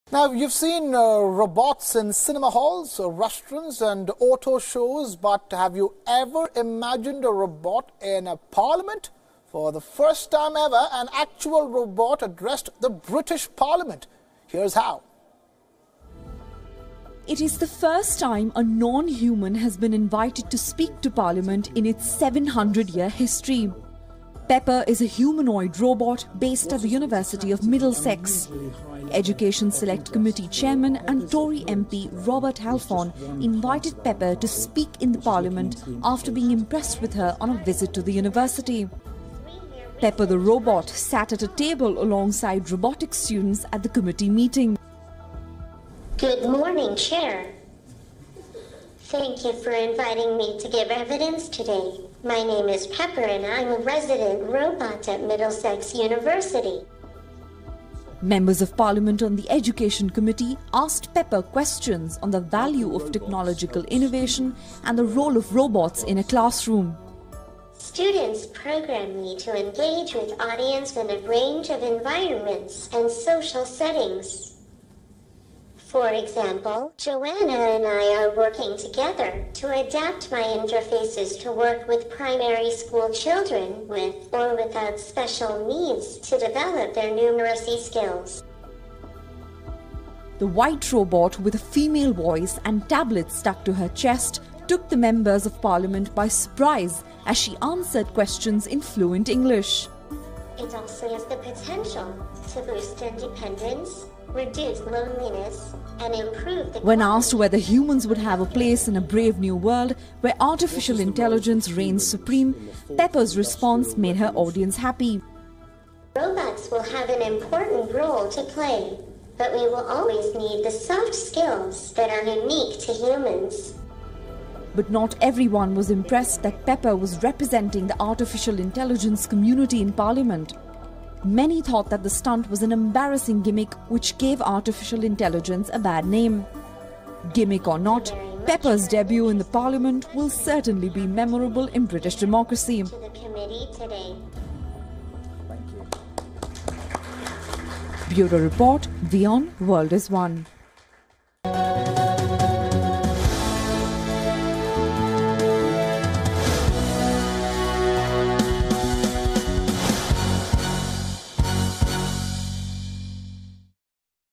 Robot makes speech in British Parliament
This segment of WION brings to you latest updates. Robot makes speech in British Parliament. Pepper the robot answers MP's questions. Watch this video to know more.